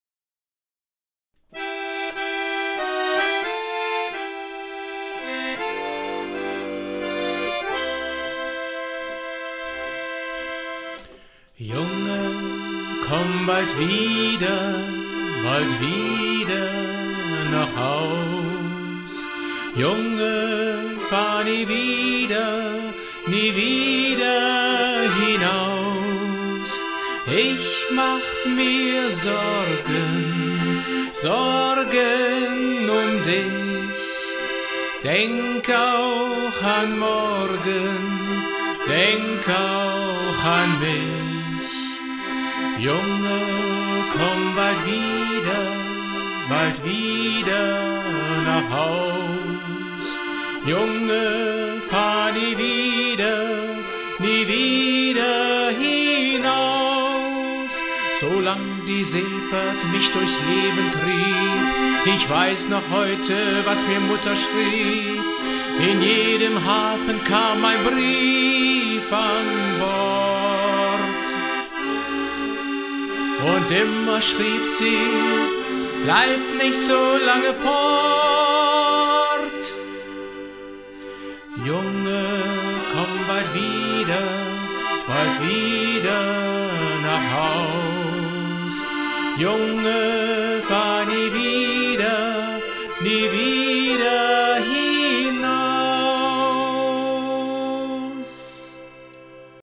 Multiinstrumentalist mit Gesang • Akkordeon mit Gesang • Künstlerportal artistenfuerdich.de
Als talentierter Sänger und Multiinstrumentalist tritt er unabhängig auf und kann Ihre Veranstaltung ganz allein begleiten.

Er spielt ausschließlich akustisch, ohne die Verwendung von Midi-Files im Hintergrund. Jeder Ton, der erklingt, stammt von ihm persönlich, und darauf legt er großen Wert!